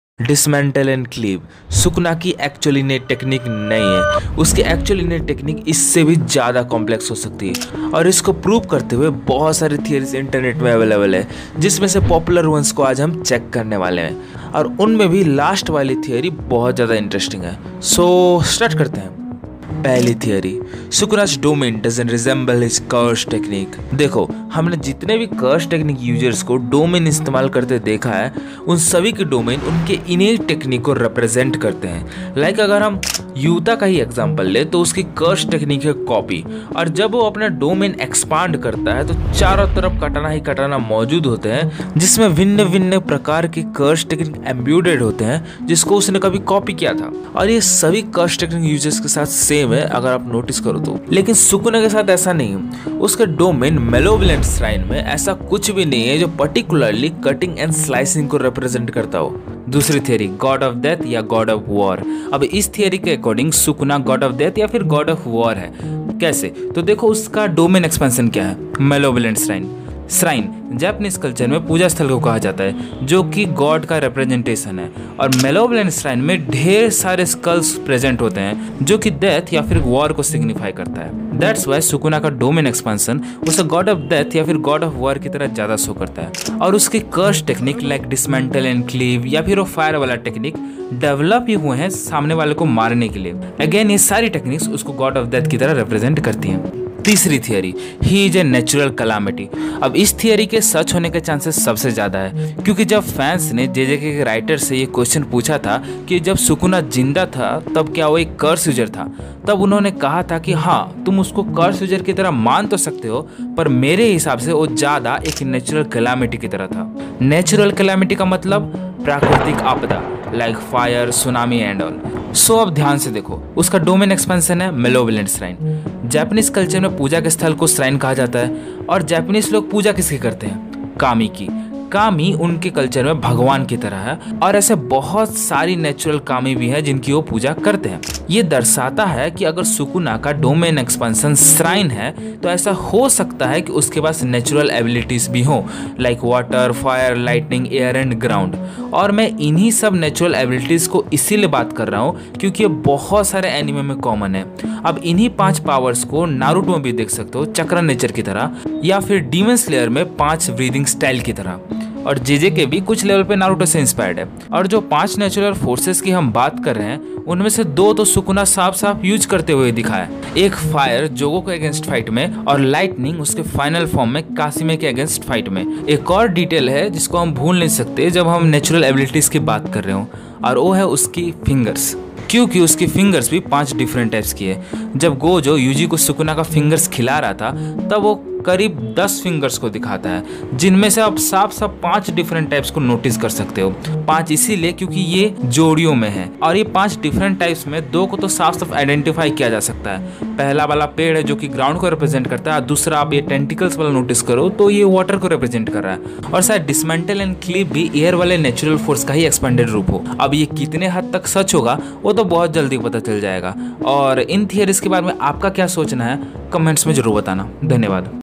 SUKUNA'S CURSE TECHNIQUE REVEALED
This video is a theory analysis of a popular anime called jujutsu kisen. In which I am explaining what can be the possible curse TECHNIQUE of SUKUNA the king of CURSE